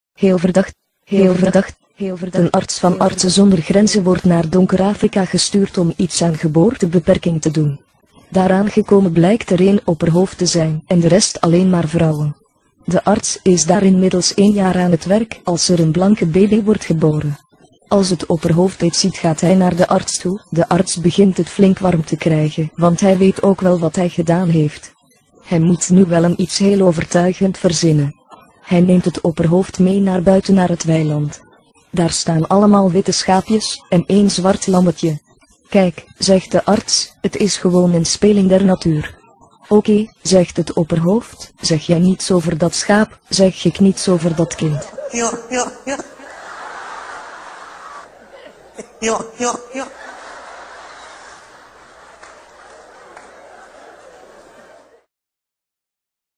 Heel verdacht !!!!
Een arts van artsen zonder grenzen wordt naar donker Afrika gestuurd om iets aan geboortebeperking te doen. Daar aangekomen blijkt er één opperhoofd te zijn en de rest alleen maar vrouwen. De arts is daar inmiddels één jaar aan het werk als er een blanke baby wordt geboren. Als het opperhoofd dit ziet gaat hij naar de arts toe, de arts begint het flink warm te krijgen want hij weet ook wel wat hij gedaan heeft. Hij moet nu wel een iets heel overtuigend verzinnen. Hij neemt het opperhoofd mee naar buiten naar het weiland. Daar staan allemaal witte schaapjes, en één zwart lammetje.
"Kijk", zegt de arts, "het is gewoon een speling der natuur."
"OK", zegt het opperhoofd, "zeg jij niets over dat schaap, zeg ik niets over dat kind..."